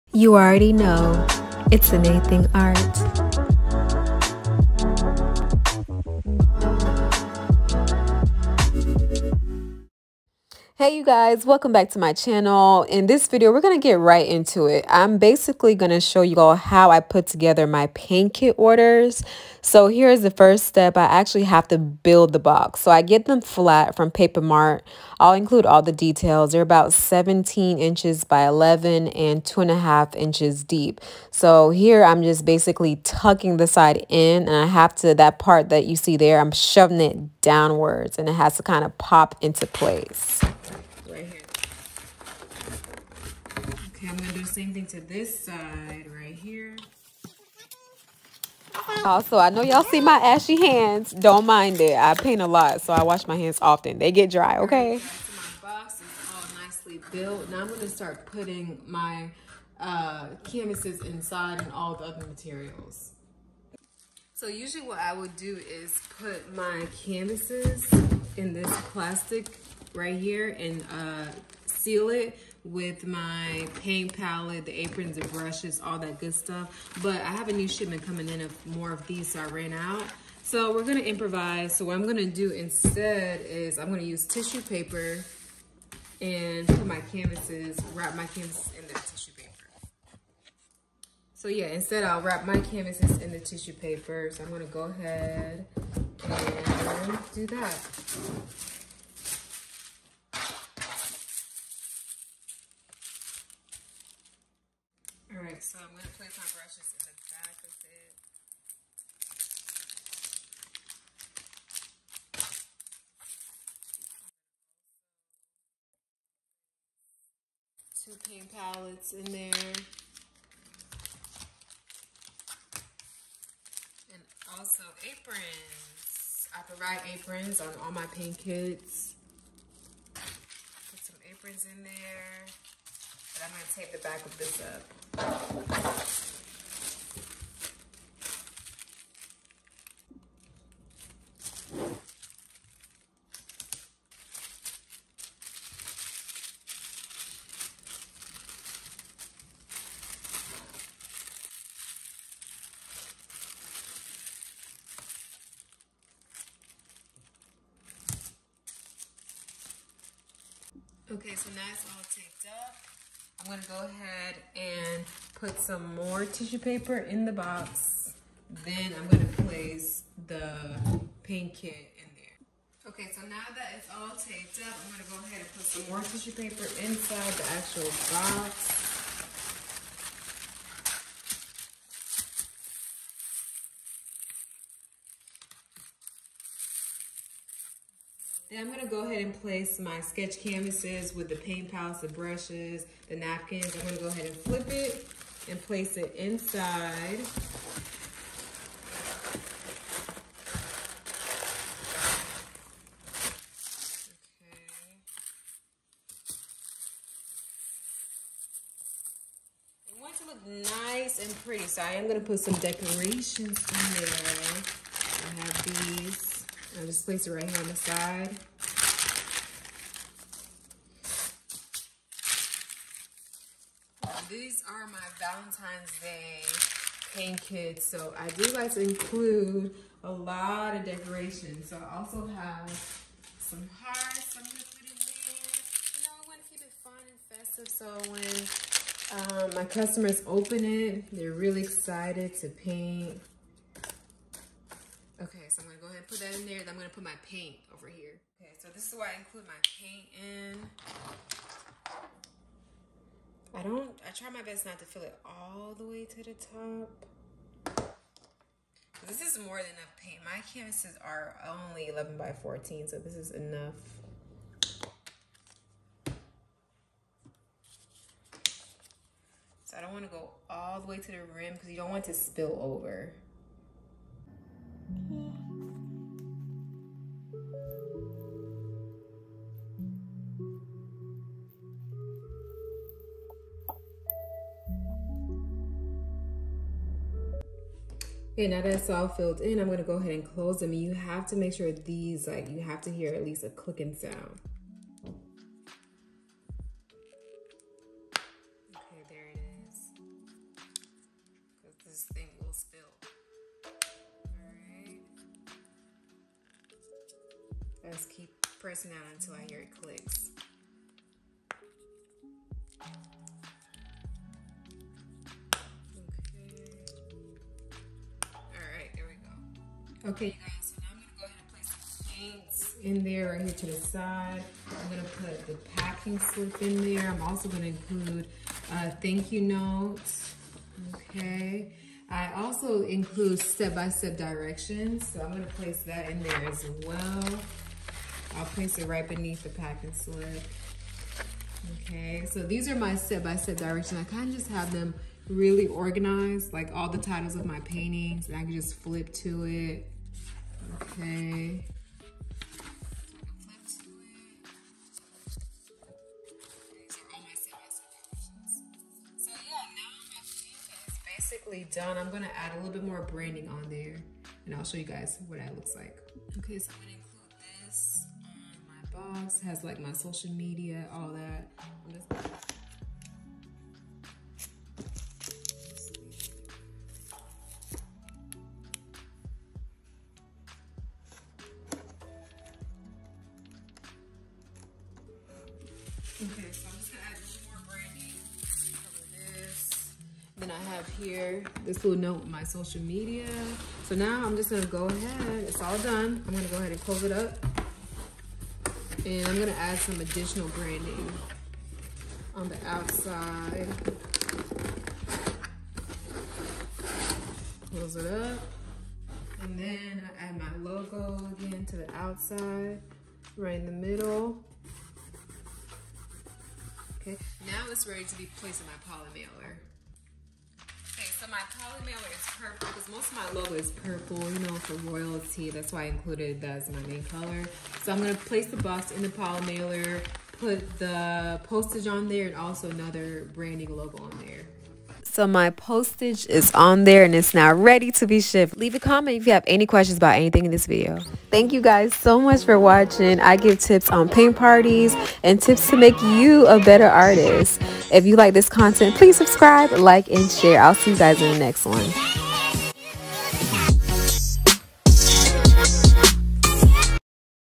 How to Pack a Paint Kit Order| Sip and Paint Kit| DIY Paint Party
Are you struggling with something Art related that I could help you with?

Paint Container Strips used in this video⇩

White Box 17x11x2 ⇩
(Cheaper and faster)

Clear Bag to Place Canvases In⇩
you could also place art prints in these. I use these for art shows too.